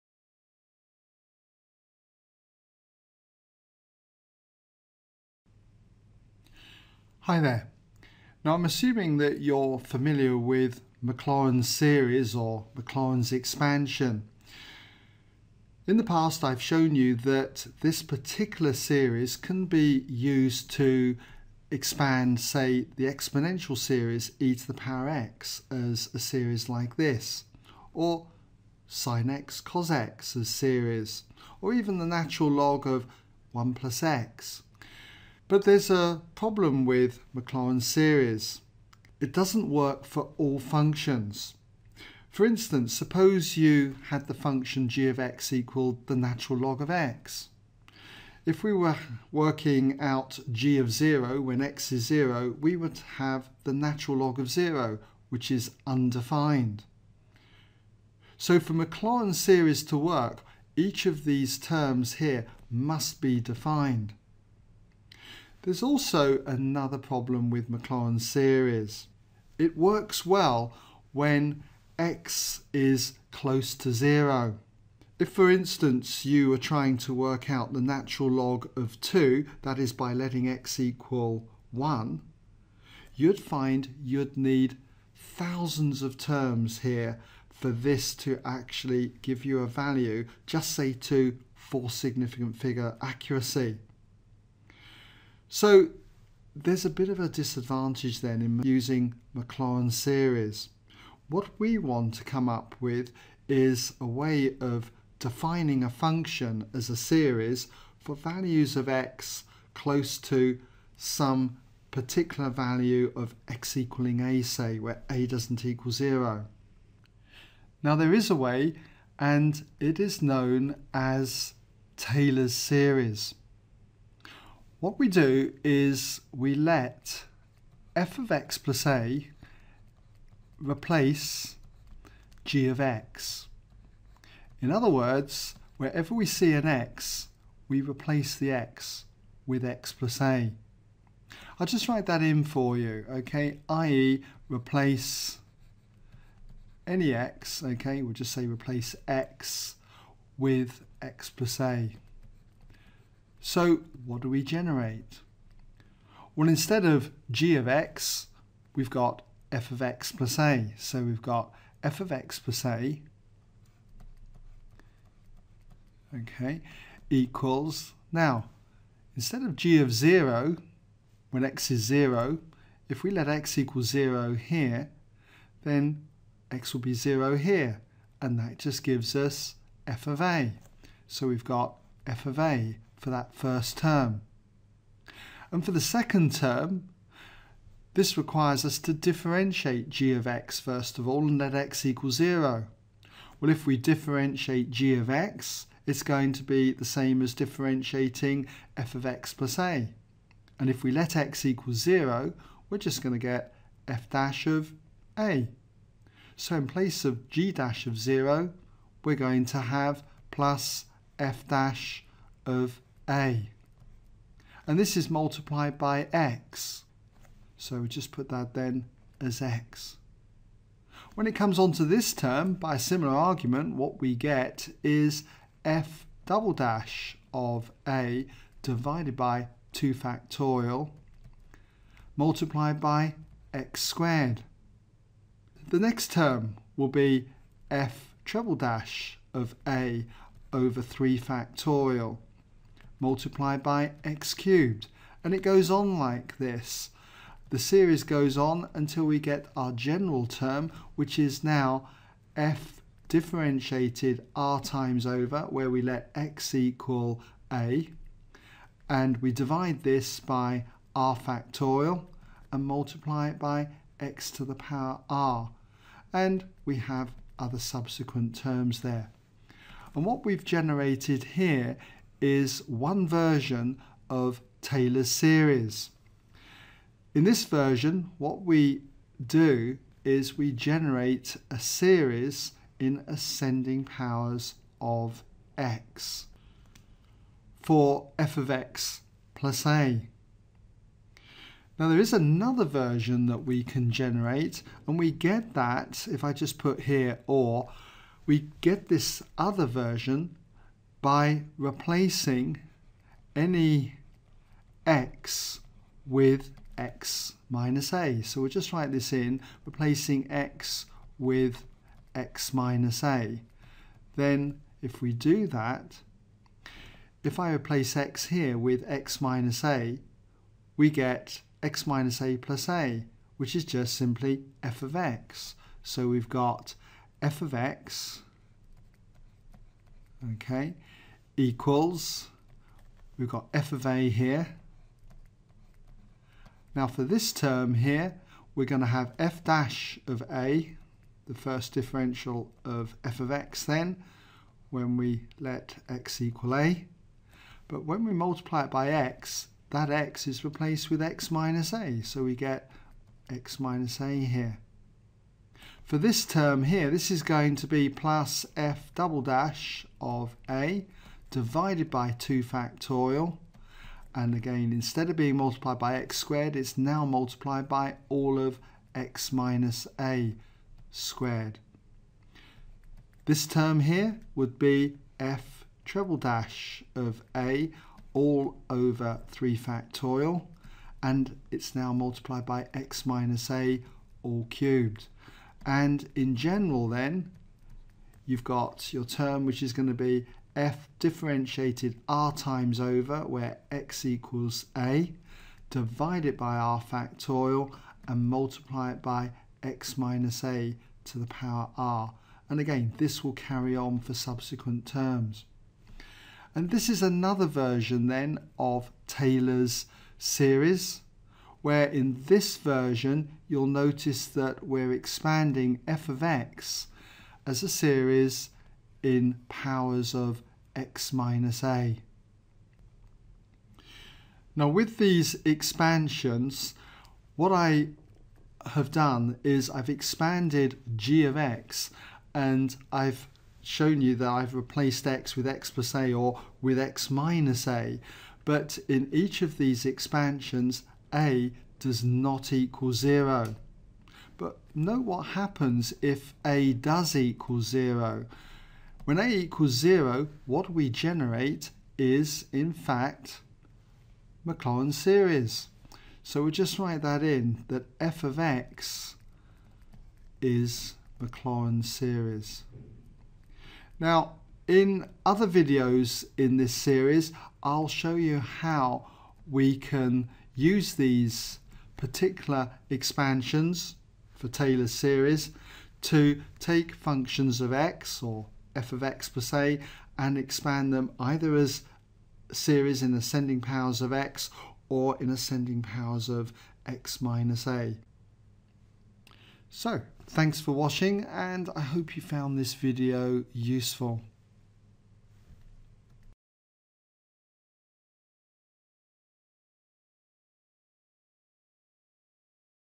Taylor's Series Expansions - Derivation : ExamSolutions Maths Revision
Revising derivation in Taylor's series expansion.

for the index, playlists and more maths videos on Taylors series and other maths topics.